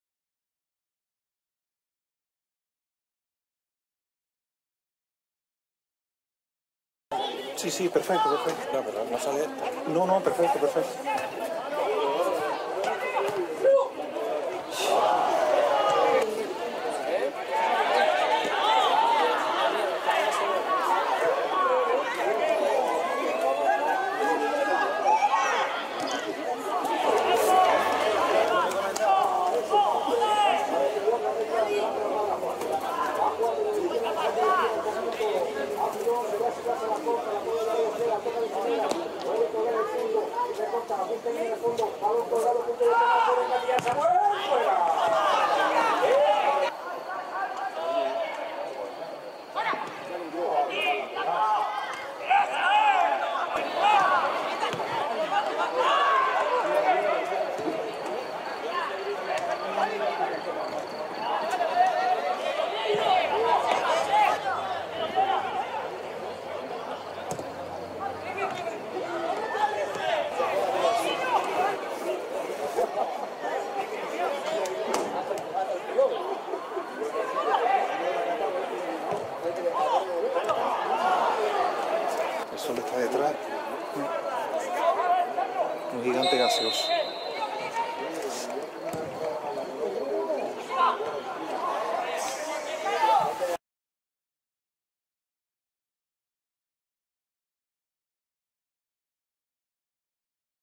8 oportunidades de gol de la UD Telde 15 4 2016
8 oportunidades de gol de la UD Telde. Partido de fútbol UD Telde-Real Sporting San José. Tercera División Canaria. Temporada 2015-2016. Jornada 34. 15/4/2016. Campo de Fútbol Municipal Pablo Hernández. Telde. Resultado final: 3-0.